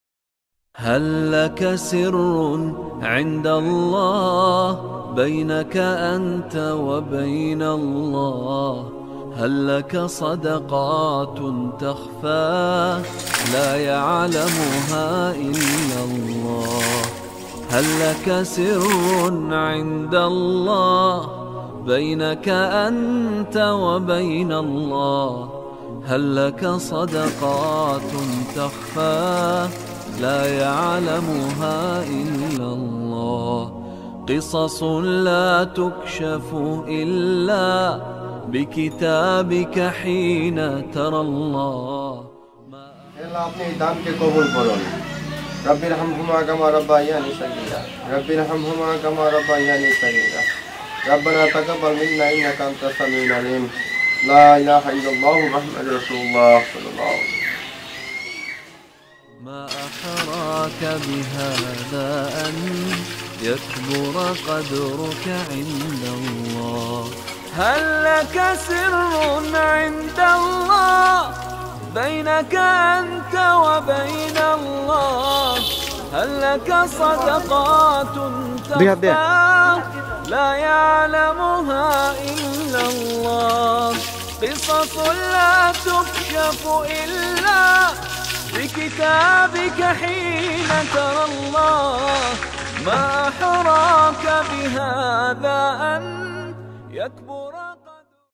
EMİNE HOCA HAYRINA BANGLADEŞ DE BİR SU KUYUSU AÇILDI.ALLAH KABUL EYLESİN.
‎Osman Yıldırım kişisinden video